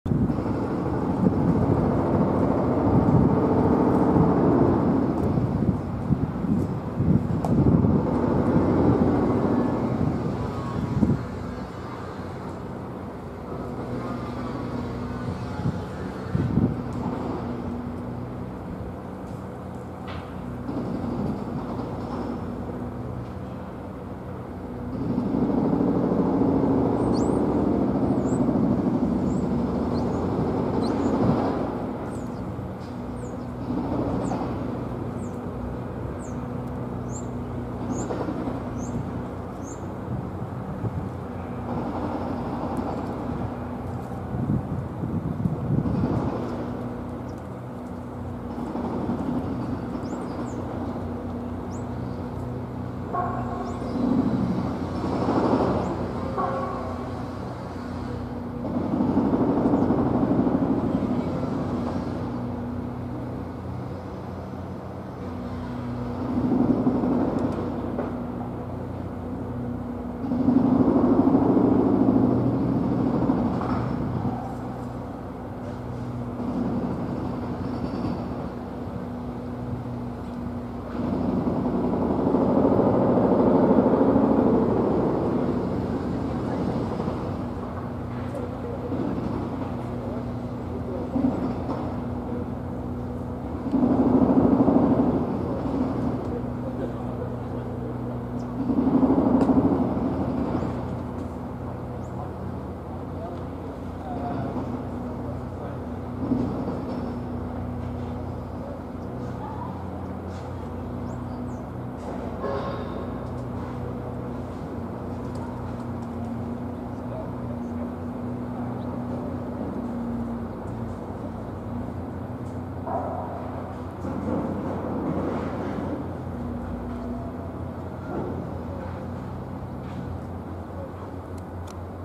Christchurch Cathedral #2 (2014-08-11-14:23)
Christchurch Cathedral from the west side. Recorded 2014, 11th of August at 2:23pm.